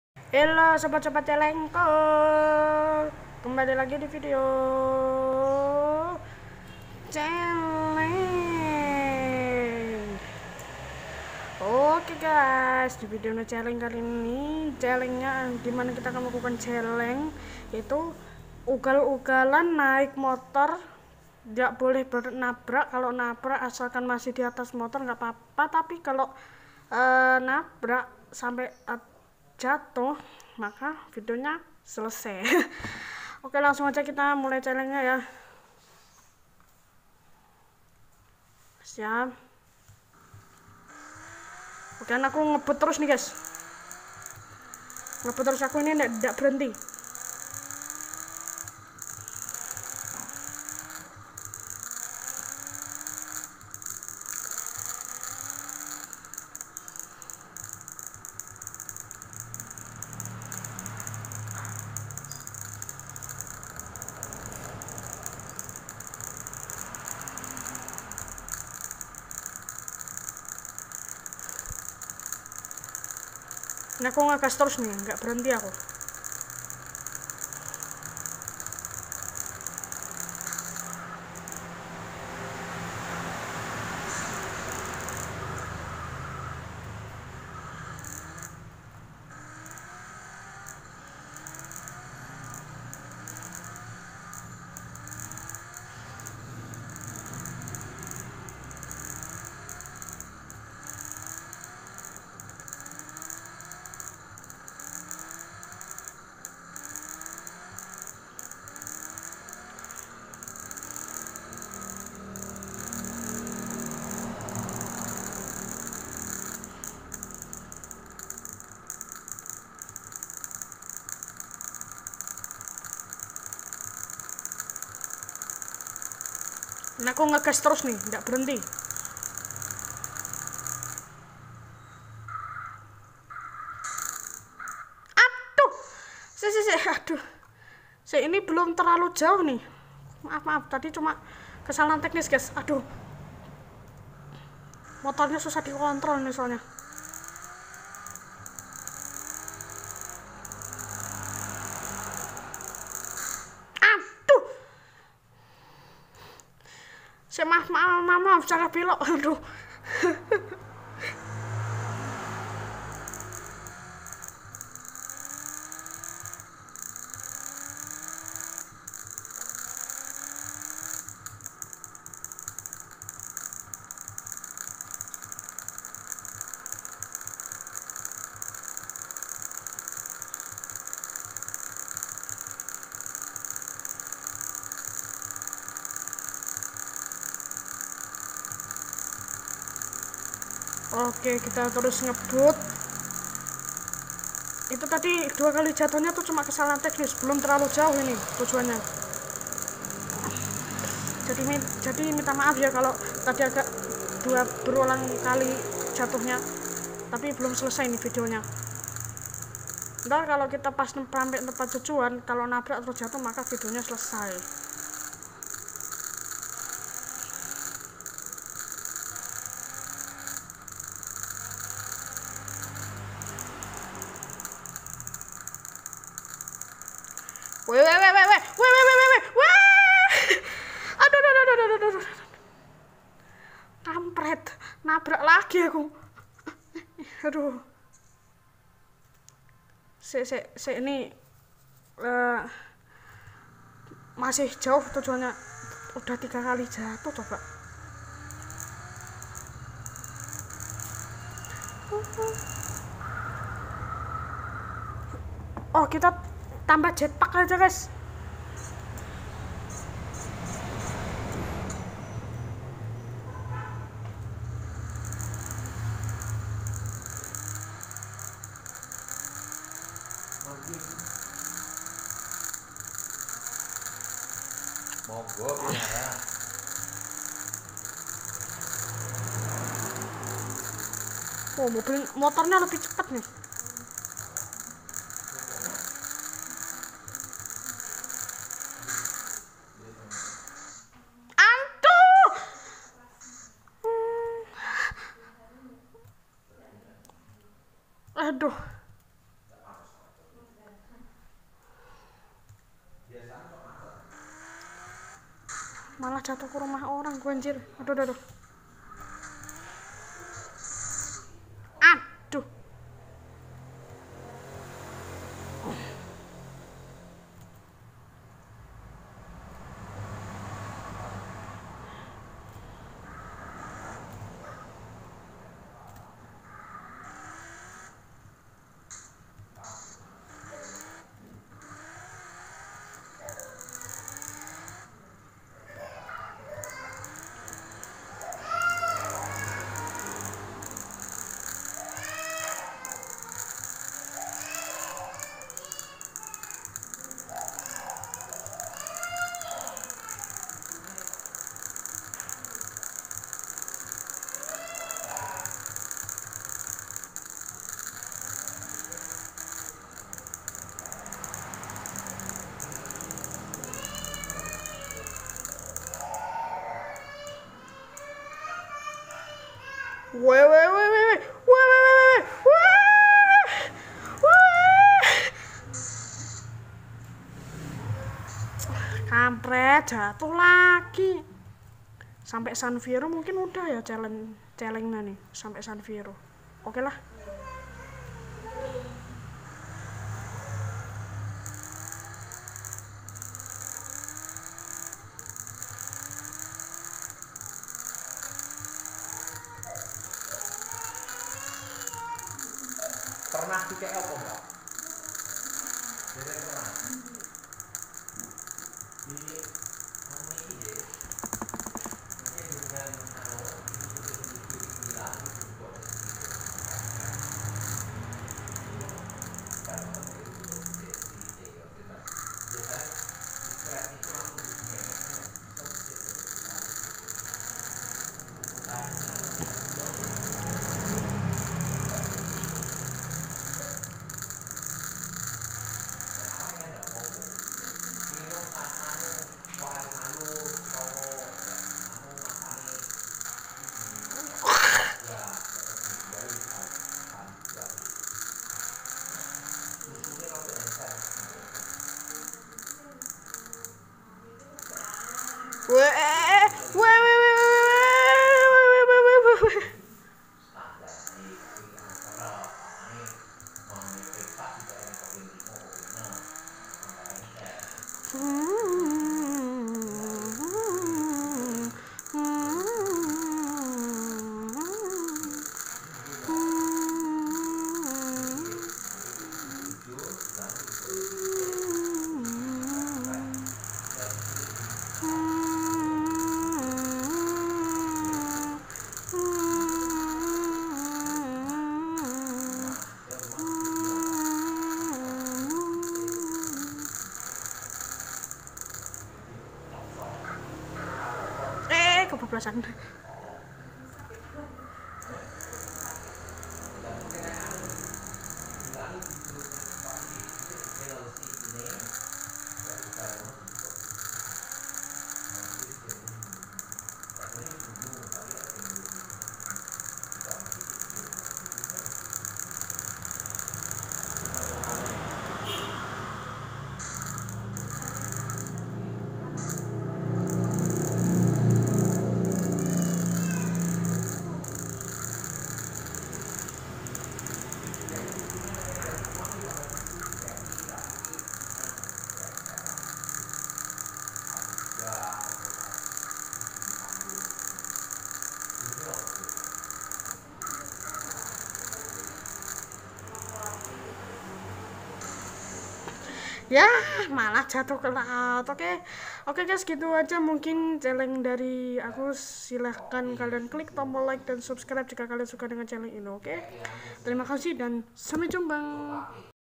Naik motor gass poll rem blonk kebut kebutan kalo sampe terjatuh dari motor videonya selesai
Celenk by:kawan kita